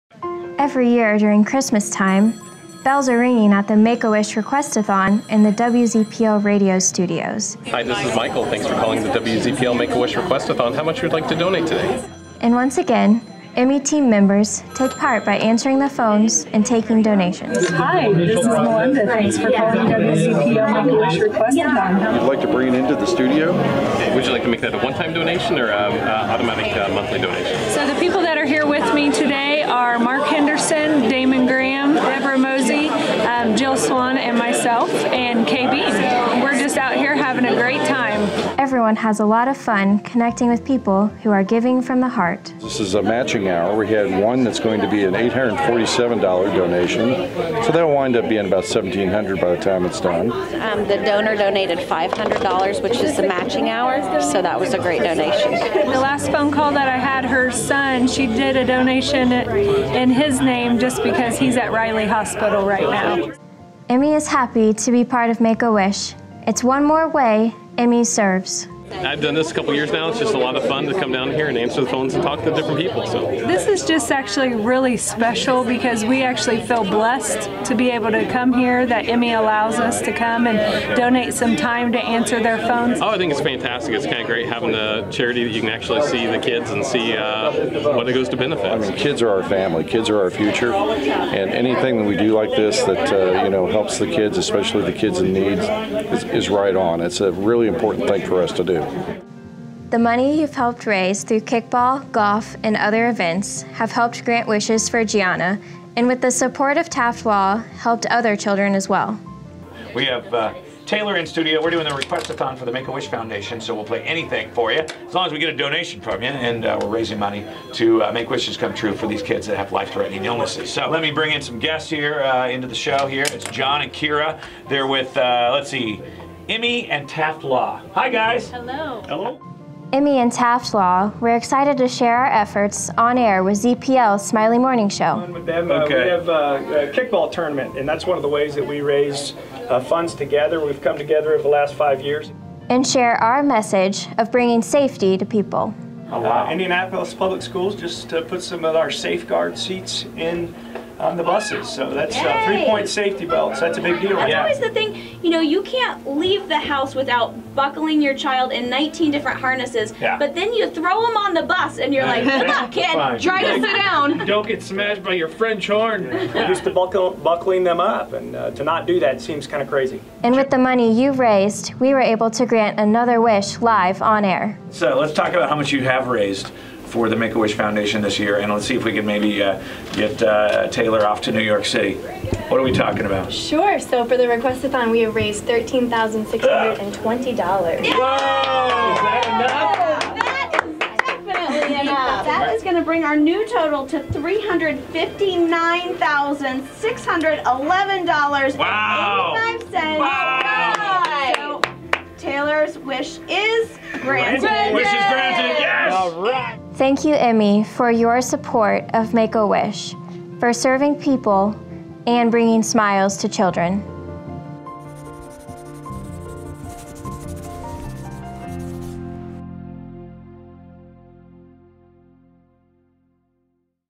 IMMI Serves Make a Wish WZPL Request-a-thon 2016
IMMI Team Members take time to serve by answering the phones and taking donations at the 2016 Make-a-Wish WZPL Request-a-thon.